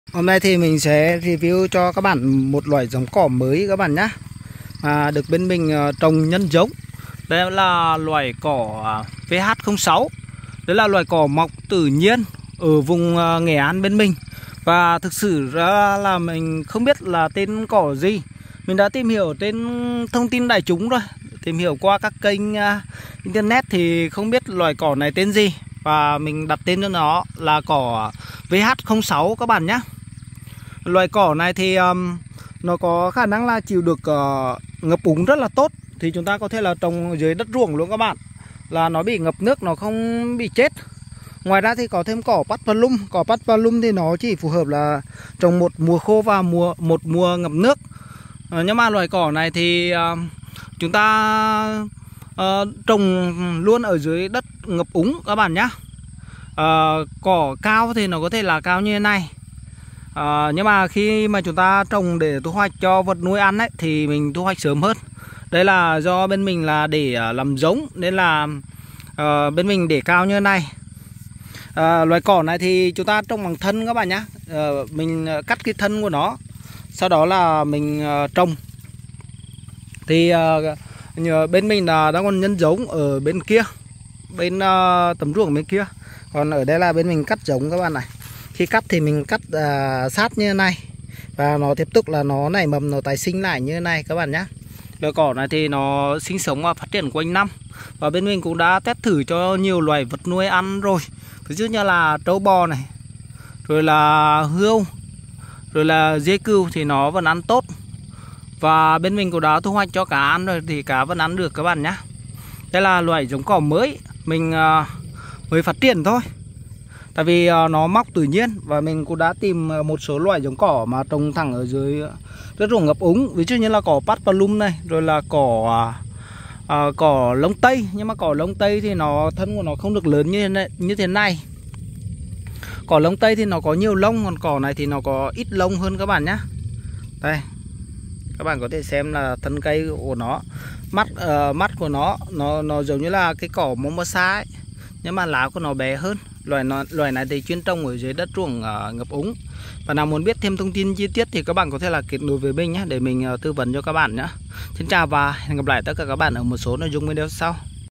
Giống Cỏ Mới Chuyên Trồng Ruộng Ngập Nước
Cỏ Chuyên trồng dưới ruộng ngập nước

Để được tư vấn về Giống Cỏ

Giống cỏ mới chuyên trồng trên ruộng ngập nước được gọi là cỏ lục bình, cỏ mồn (Paspalum dilatatum). Đây là một loại cỏ thích nghi tốt với điều kiện ngập nước và có khả năng chịu được môi trường đất nhiều độ mặn.

Cỏ lục bình, cỏ mồn có khả năng sinh trưởng mạnh mẽ và phát triển rễ sâu, giúp cung cấp độ bền và sức mạnh cho ruộng ngập nước. Nó cũng có khả năng chịu được tác động của nước mặn và nước lợ, làm cho nó trở thành một lựa chọn phổ biến cho việc trồng trên ruộng ngập nước.

Đặc điểm của cỏ lục bình, cỏ mồn  bao gồm lá xanh mướt và rễ mạnh mẽ. Nó có thể chịu được tác động của nước mặn và có khả năng tái sinh nhanh chóng sau khi bị chập tử. Cỏ lục bình cũng có khả năng cạnh tranh tốt với các loại cỏ khác trong môi trường ngập nước.

Việc trồng cỏ lục bình trên ruộng ngập nước có thể giúp cải thiện chất lượng đất và kiểm soát sự phát triển của các loài cỏ gây hại. Nó cung cấp một bề mặt thảm cỏ đồng nhất, giúp hạn chế sự mất nước do hơi và gió. Ngoài ra, cỏ lục bình cũng có khả năng giảm sự xói mòn đất do dòng nước, làm cho nó trở thành một lựa chọn hữu ích cho việc bảo vệ bờ ruộng.

Tuy nhiên, trước khi trồng cỏ lục bình, cỏ mồn  bạn nên tìm hiểu kỹ về yêu cầu và phương pháp chăm sóc của nó để đảm bảo việc trồng thành công trên ruộng ngập nước.